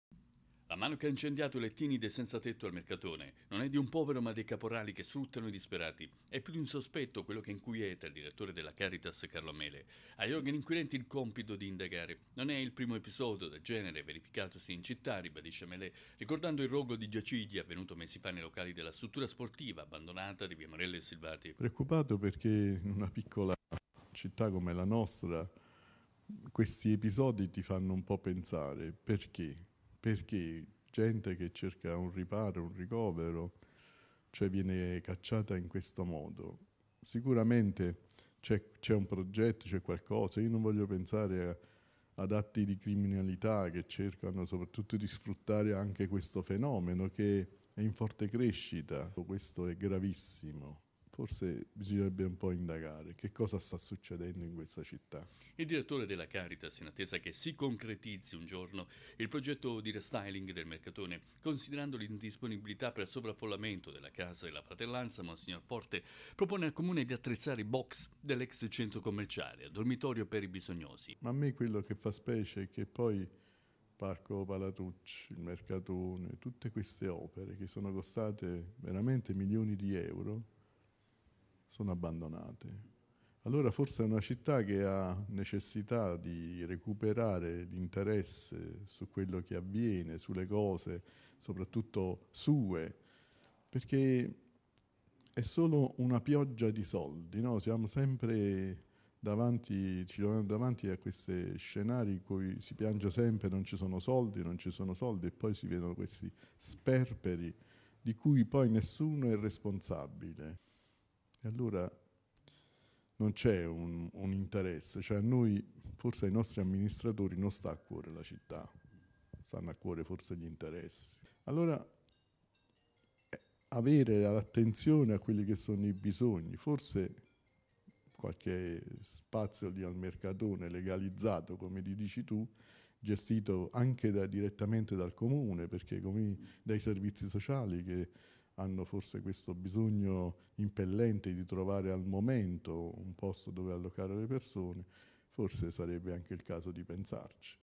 Fiamme ai giacigli dei senzatetto, la mano dei caporali che sfruttano i disperati
L'ombra del caporalato dietro il rogo di lettini per clochard al mercatone. Il direttore della Caritas chiede all'autorità giudiziaria di fare luce sul caso. Intanto Calo Mele propone al comune di attrezzare i box dell'ex centro commerciale a dormitorio.